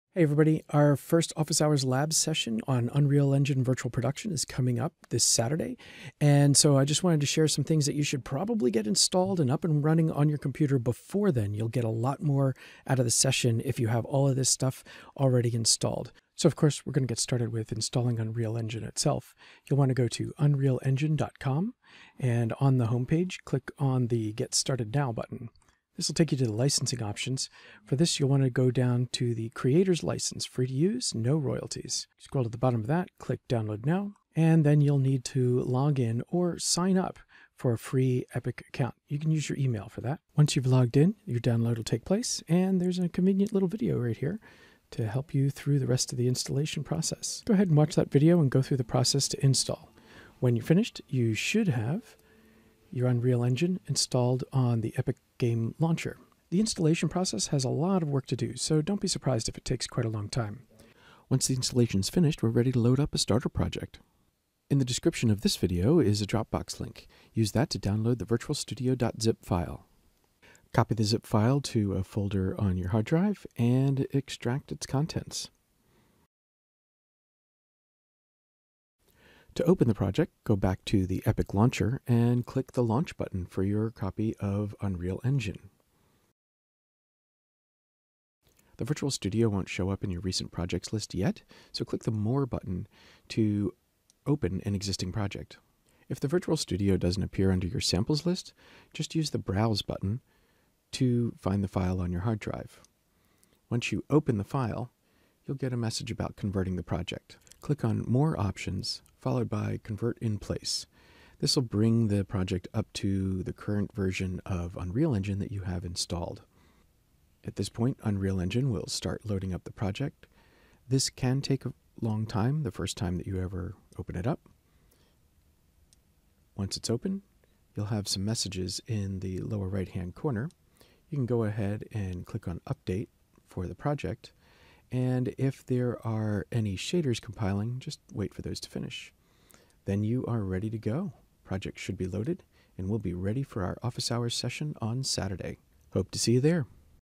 Office Hours Labs: Week 01 Prep
The first, live Office Hours Labs session is coming up on Saturday, April 3, 2021. To get the most from the session, you should install Unreal Engine and the VirtualStudio demo project onto your computer in advance. This video will walk through the process.

You can use this Dropbox link to download the Virtual Studio.zip file containing the demo studio project: